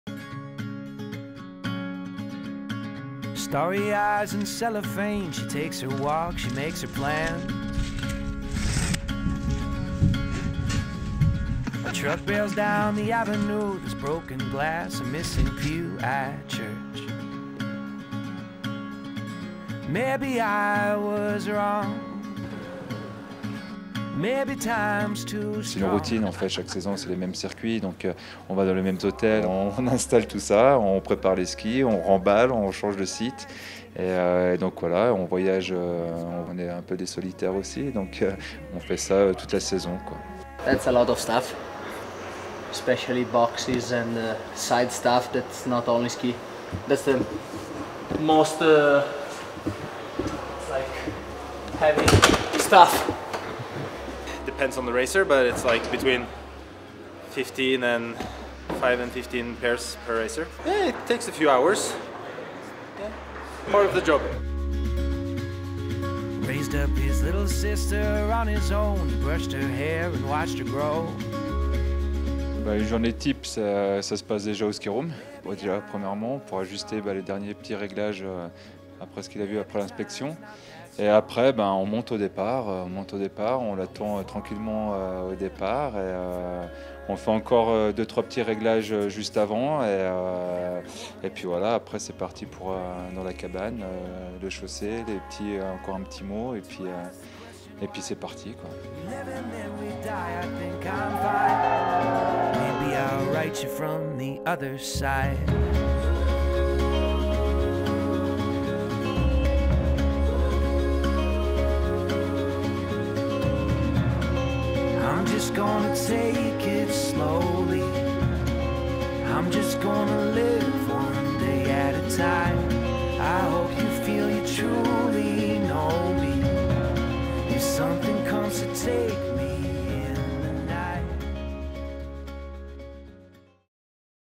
The World Cup Tour, a logisitcal challenge | FIS Alpine
Travelling from one venue to the other with all the equipement a team needs can be a real logistical challenge. We followed the teams in their transfer from Zinal to the World Cup in Val d'Isère, and the least we can say is that these guys are dedicated to their team and their athletes. Thanks to all our staff members for the hard work!